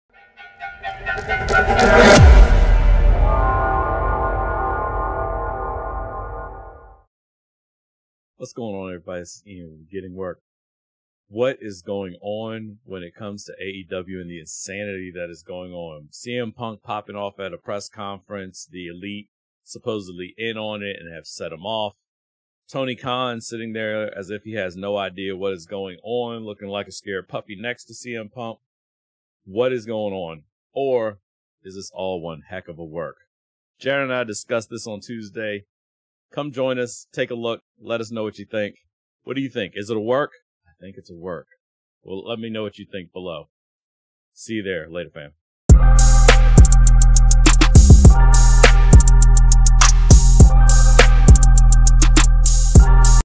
AEW Insanity - Watch our longer video
Check out our AEW discussion and let us know what you think! CM Punk, The Elite, Tony Khan and more talked about. How crazy and strangely wonderful from a fan side all of this is!